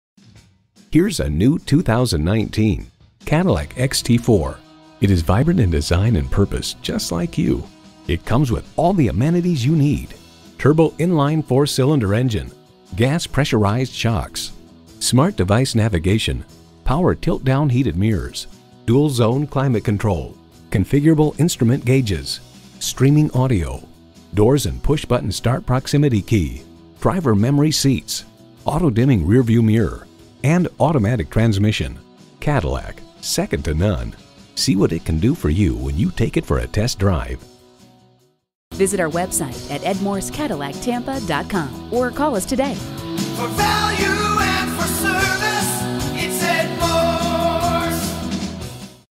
New 2019 Cadillac XT4 Tampa FL St. Petersburg, FL KF176183 - SOLD
Year: 2019
Make: Cadillac
Model: XT4
Trim: Sport
Engine: 2l 4 Cylinder Turbocharged
Transmission: FWD Automatic
Color: Black Metallic
Interior: Wheat
Stock #: KF176183
VIN: 1GYFZER45KF176183

Address:
101 East Fletcher Avenue